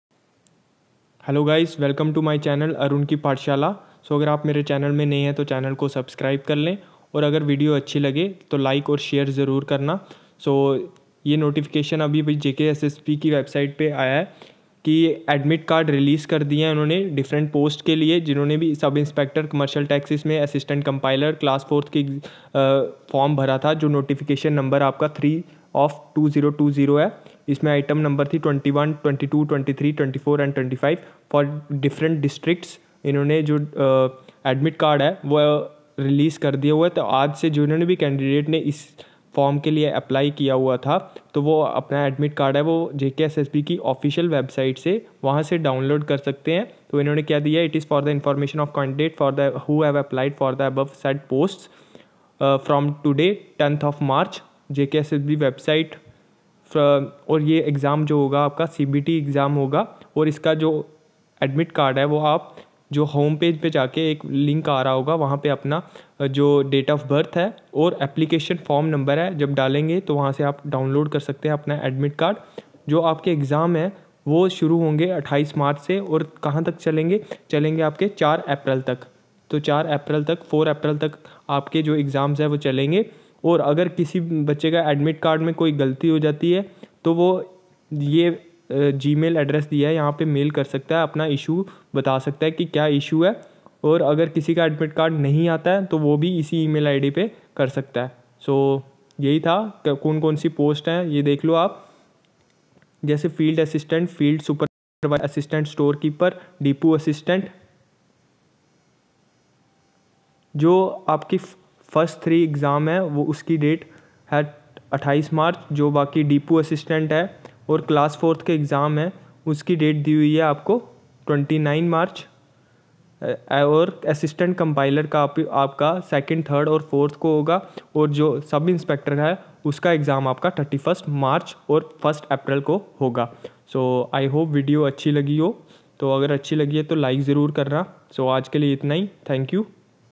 JKSSB Released Admit Cards for Notification 03/2020.Kashmiri Migrants
JKSSB Released Admit Cards for class IV,SI, assistant compiler and various other posts